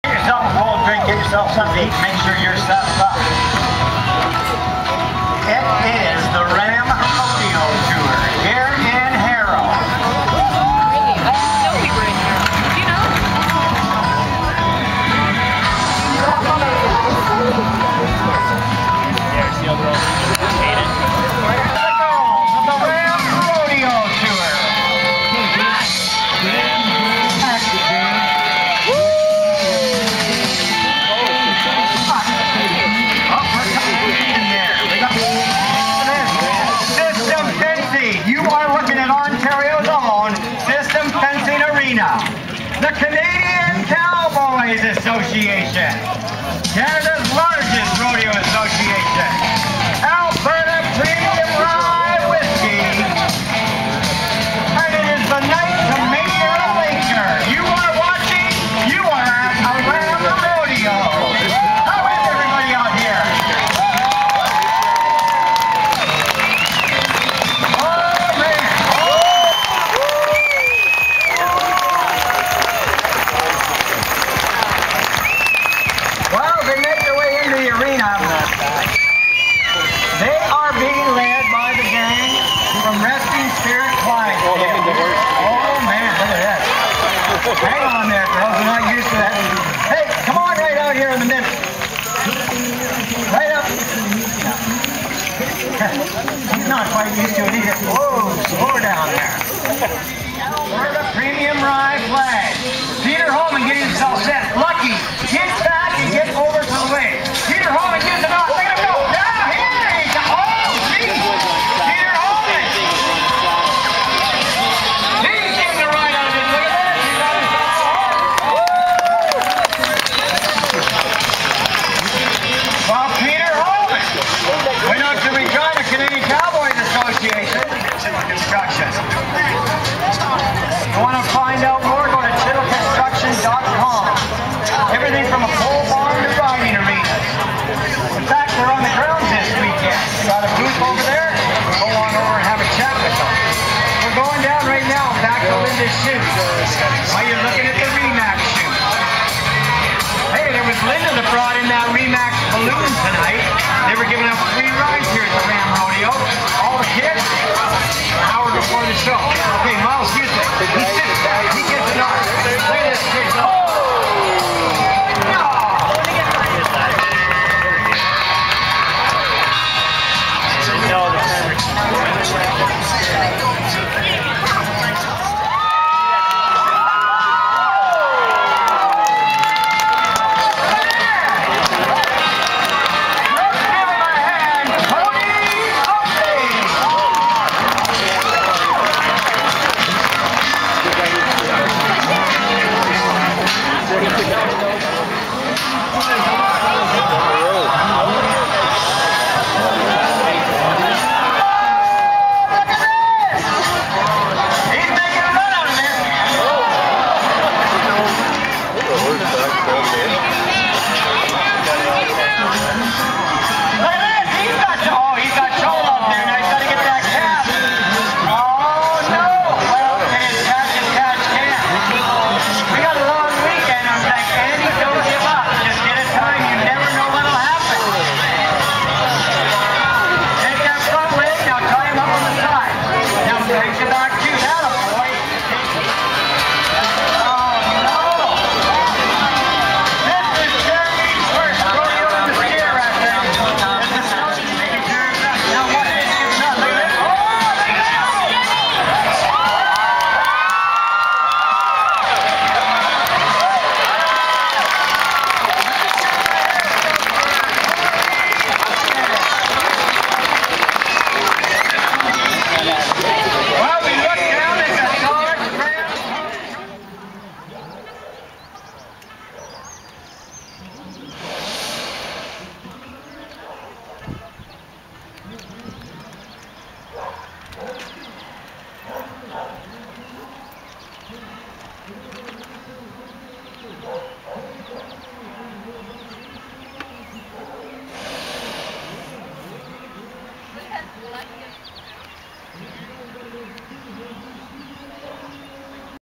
Harrow Rodeo - The 2011 Country 95.9 Rock N' Horse Fest Part 1
The 'Country 95.9 Rock 'N Horse Fest, billed as the the largest Country Music Concert in south western Ontario was held on the Victoria Day Weekend of May 20th, 2011 to Monday May23, 2011. The four day festival consisted of the RAM Rodeo Tour and many other Equestrian competitions which showcased some of the great talent of the many cowboys and cowgirls of all ages who competed in a fun filled family event. Musical entertainment consisted of headliners Crystal Gage who opened for popular Canadian country music artist Aaron Lines with the following days providing many more quality musicians.

The event was also held to help raise awareness and support for the Breast Cancer Society of Canada.